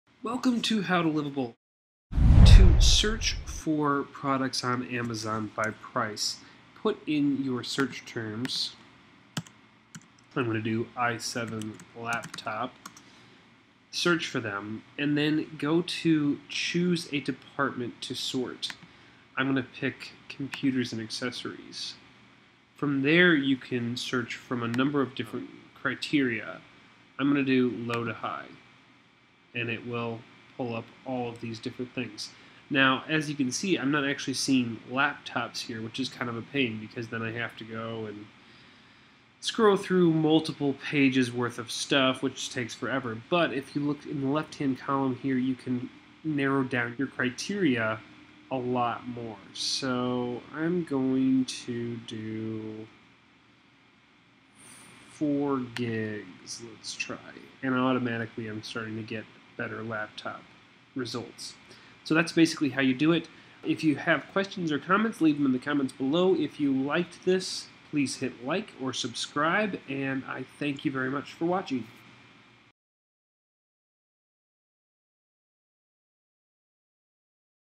How to Search by Price on Amazon
- Put in your search terms and go to Choose a Department to sort
- Choose the department that you are searching for
- You can search from different criteria (Relevance, Most Popular, Price: Low to High, High to Low, Avg. Customer Review)
- Select Price: Low to High
- Select Computer RAM Capacity 4GB & under